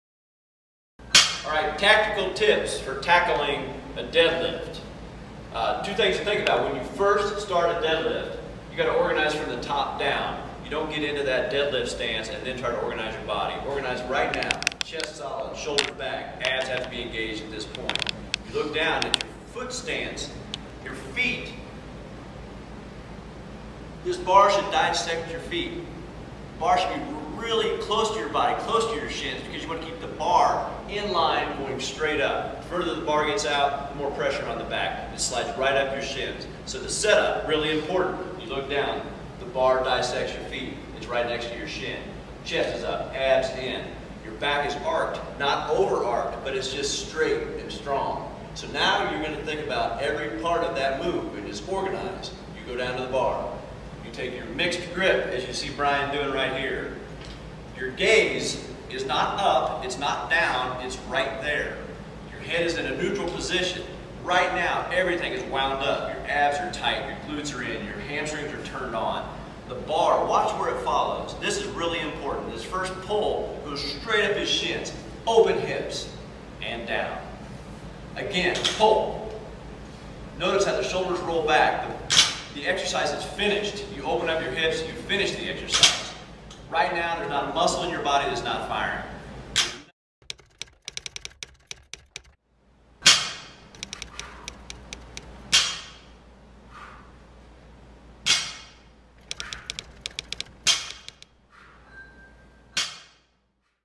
:: Deadlift technique tips with crossfit coach John Benz ::
Crossfit on 18th coach, John Benz, explains proper technique for deadlift exercise.

Kansas City. August 2012.